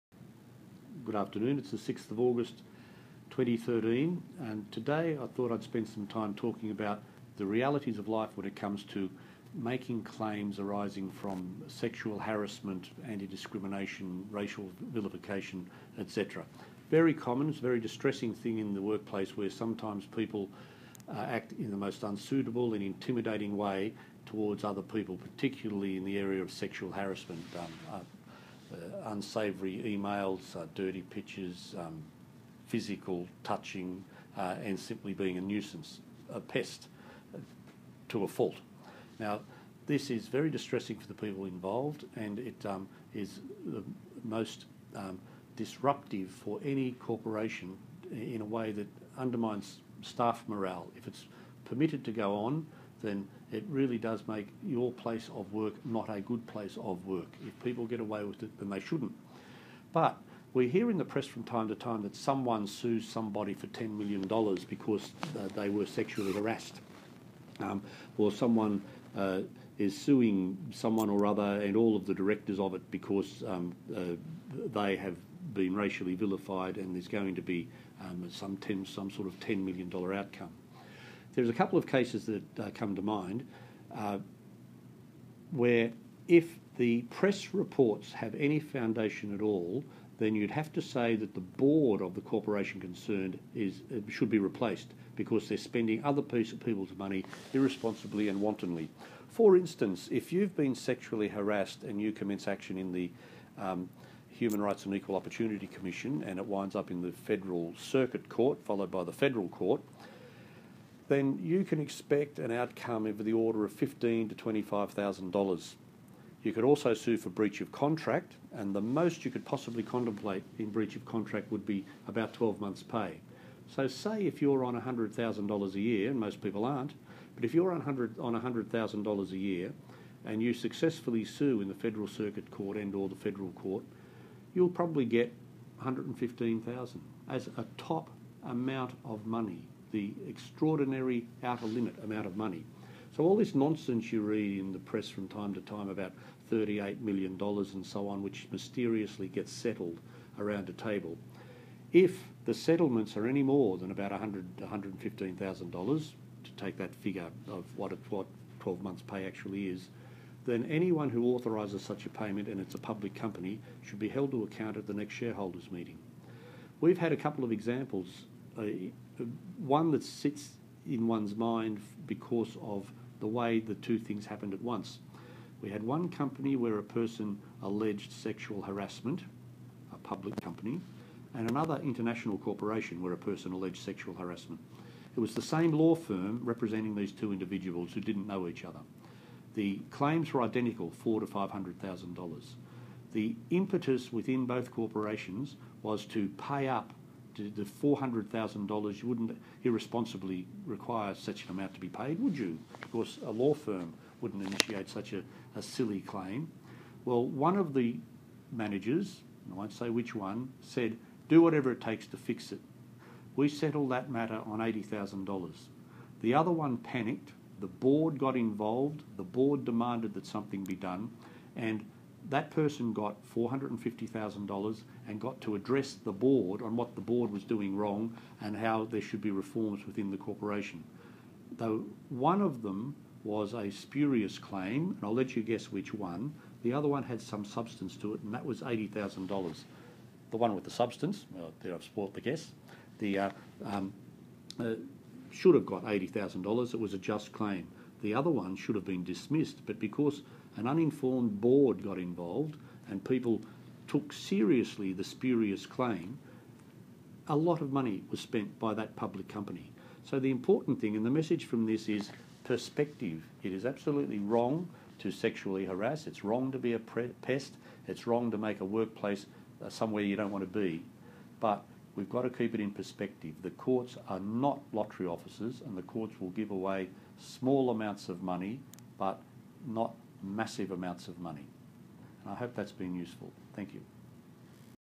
Craig Thomson's Lawyer Chris McArdle,
the reality of making a discrimination claim